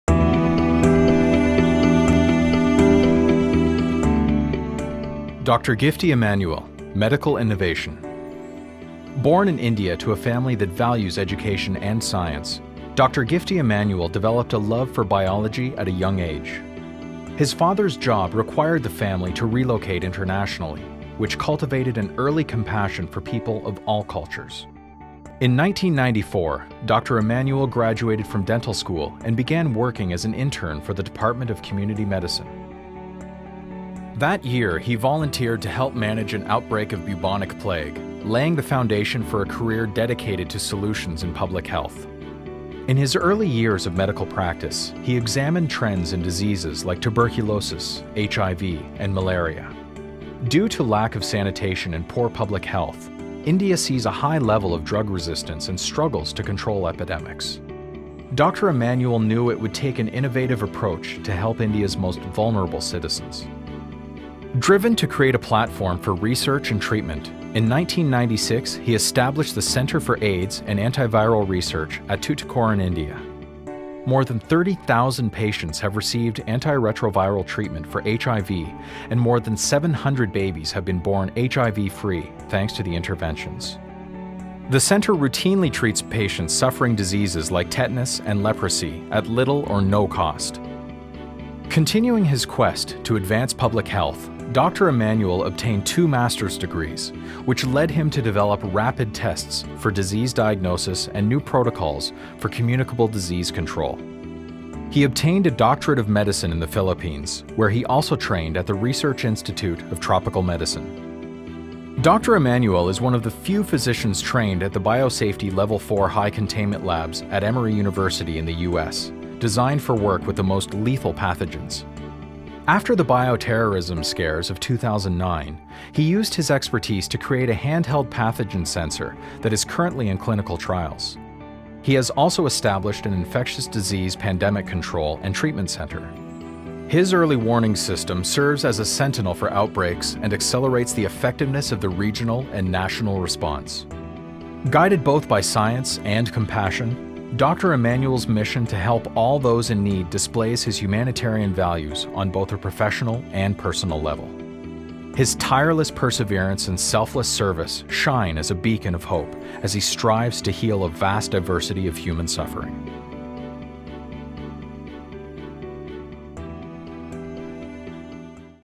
2010 JCI TOYP Honoree Dr. Gifty Immanuel
For his extraordinary medical work in the field of infectious diseases and epidemics, Dr. Gifty Immanuel of Tuticorin, India was named a 2010 JCI Ten Outstanding Young Persons of the World (JCI TOYP) recipient in the category of medical innovation.

After volunteering to help manage an outbreak of the bubonic plague in India, Immanuel began a long career studying and treating infectious diseases and epidemics. After years of seeking support for his vision of an infectious diseases research and treatment center, Immanuel established the Center for AIDS & Antiviral Research in Tuticorin, India in 1996. Since that time, the center has treated nearly 30,000 HIV patients, as well as victims of other diseases including tuberculosis, leprosy, malaria and typhoid free of charge or at subsidized cost.